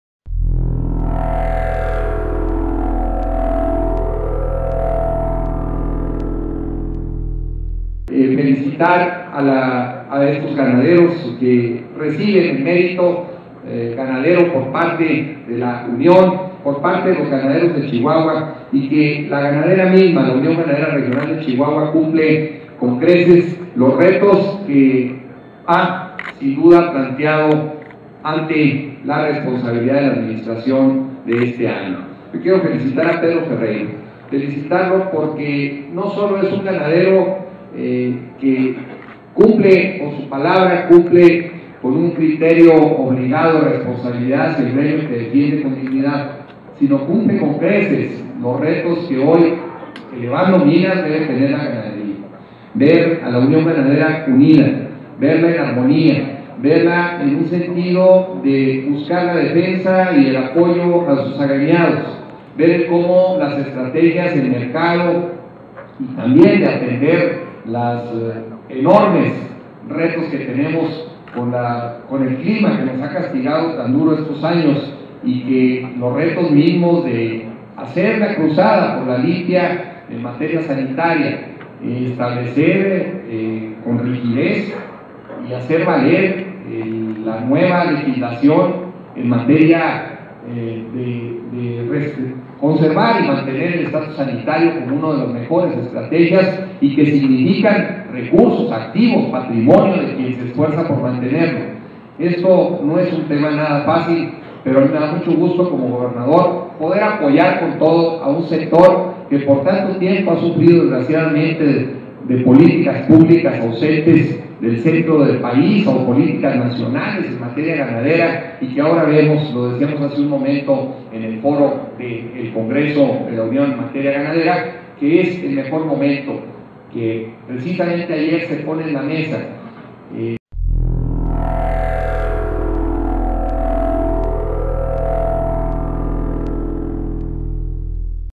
Encabezó Duarte la LXXVII Asamblea General Anual de la Ugrch
El mandatario estatal fue reconocido por Pedro Rubén Ferreiro, presidente de la Unión Regional Ganadera de Chihuahua